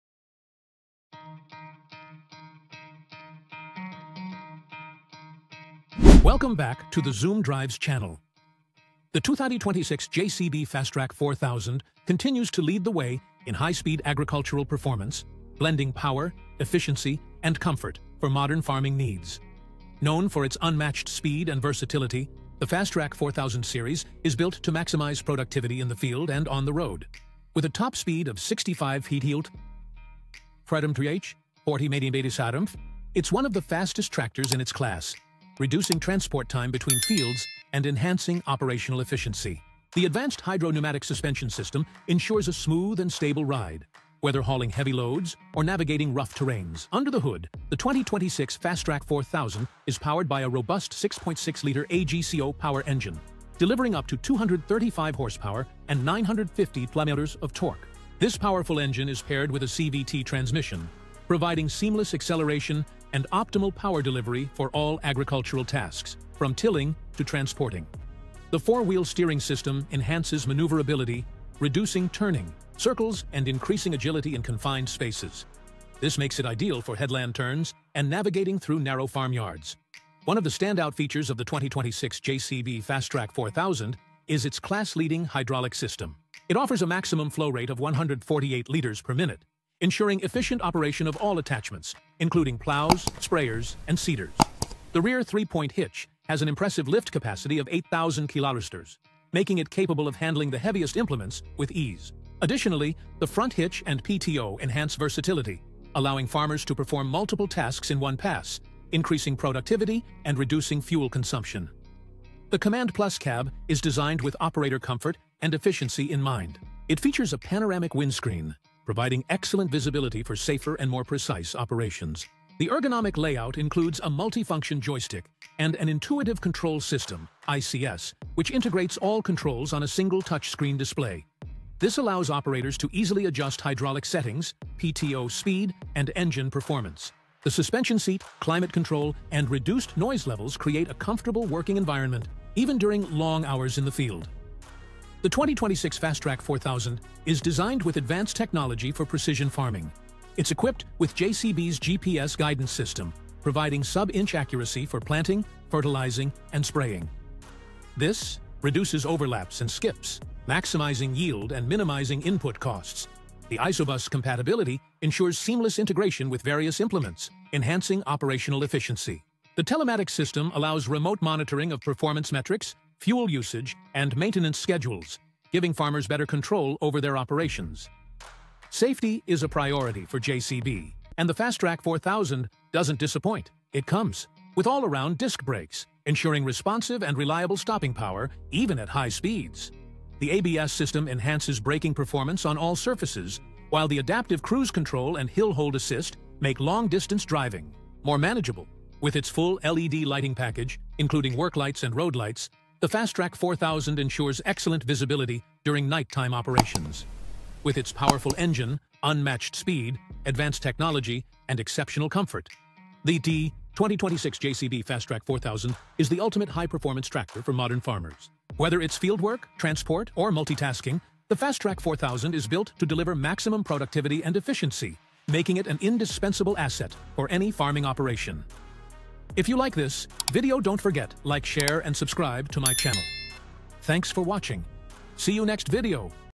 All New 2026 JCB Fastrac 4000 Review – High-Speed Tractor Redefined!
The 2026 JCB Fastrac 4000 continues to lead the way in high-speed agricultural performance, blending power, efficiency, and comfort for modern farming needs. Known for its unmatched speed and versatility, the Fastrac 4000 series is built to maximize productivity in the field and on the road. With a top speed of 65 km/h (40 mph), it’s one of the fastest tractors in its class, reducing transport time between fields and enhancing operational efficiency. The advanced hydro-pneumatic suspension system ensures a smooth and stable ride, whether hauling heavy loads or navigating rough terrains.

Under the hood, the 2026 Fastrac 4000 is powered by a robust 6.6-liter AGCO Power engine, delivering up to 235 horsepower and 950 Nm of torque. This powerful engine is paired with a CVT transmission, providing seamless acceleration and optimal power delivery for all agricultural tasks, from tilling to transporting. The four-wheel steering system enhances maneuverability, reducing turning circles and increasing agility in confined spaces. This makes it ideal for headland turns and navigating through narrow farmyards.

One of the standout features of the 2026 JCB Fastrac 4000 is its class-leading hydraulic system. It offers a maximum flow rate of 148 liters per minute, ensuring efficient operation of all attachments, including plows, sprayers, and seeders. The rear three-point hitch has an impressive lift capacity of 8,000 kg, making it capable of handling the heaviest implements with ease. Additionally, the front hitch and PTO enhance versatility, allowing farmers to perform multiple tasks in one pass, increasing productivity and reducing fuel consumption.

The CommandPlus cab is designed with operator comfort and efficiency in mind. It features a panoramic windscreen, providing excellent visibility for safer and more precise operations. The ergonomic layout includes a multi-function joystick and an Intuitive Control System (ICS), which integrates all controls on a single touchscreen display. This allows operators to easily adjust hydraulic settings, PTO speed, and engine performance. The suspension seat, climate control, and reduced noise levels create a comfortable working environment, even during long hours in the field.

The 2026 Fastrac 4000 is designed with advanced technology for precision farming. It’s equipped with JCB’s GPS guidance system, providing sub-inch accuracy for planting, fertilizing, and spraying. This reduces overlaps and skips, maximizing yield and minimizing input costs. The ISOBUS compatibility ensures seamless integration with various implements, enhancing operational efficiency. The telematics system allows remote monitoring of performance metrics, fuel usage, and maintenance schedules, giving farmers better control over their operations.